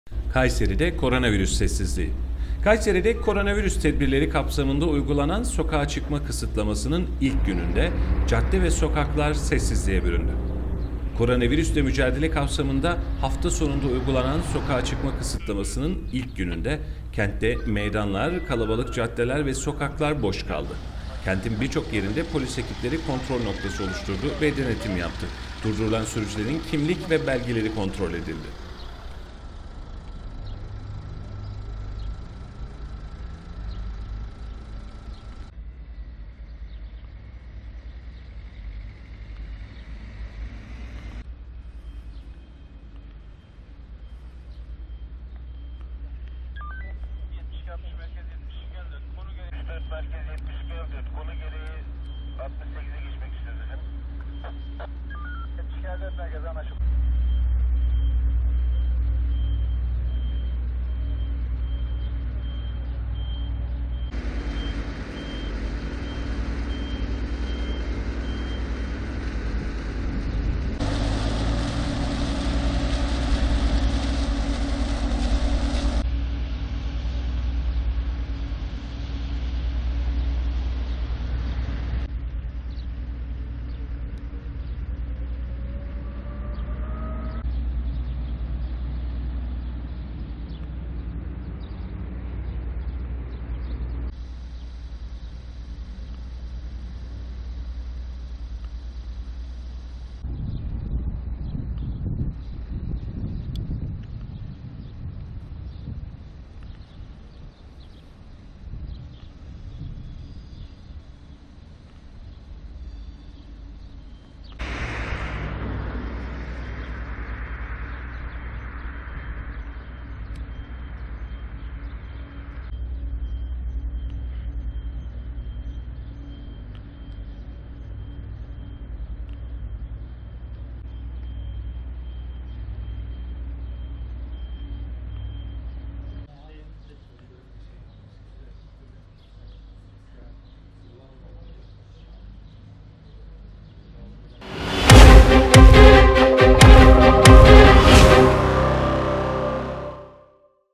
Kayseri'de 'koronavirüs' sessizliği
KAYSERİ'de koronavirüs tedbirleri kapsamında uygulanan sokağa çıkma kısıtlamasının ilk gününde cadde ve sokaklar sessizliğe büründü.

Koronavirüsle mücadele kapsamında hafta sonunda uygulanan sokağa çıkma kısıtlamasının ilk gününde kentte meydanlar, cadde ve sokaklar boş kaldı. Kentin birçok yerinde polis ekipleri kontrol noktası oluşturdu ve denetim yaptı. Durdurulan sürücülerin kimlikleri kontrol edildi, belgelerine bakıldı.